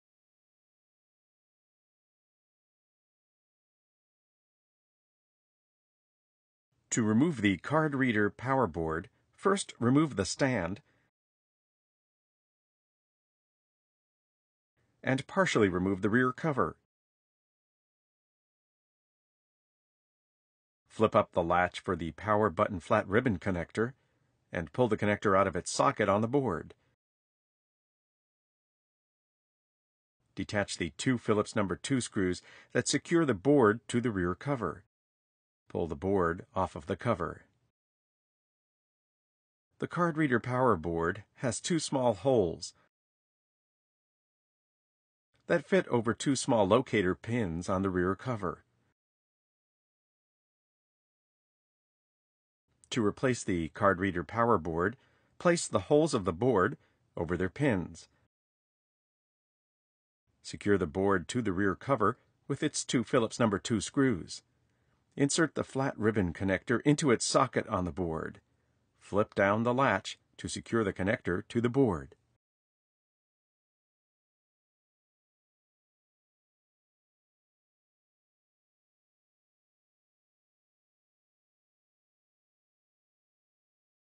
Replace the Card Reader/Power Board | HP 22-b400 All-in-One Desktop PC Series
Learn how to replace the card reader/power board for HP 22-b400 All-in-One Desktop PC series.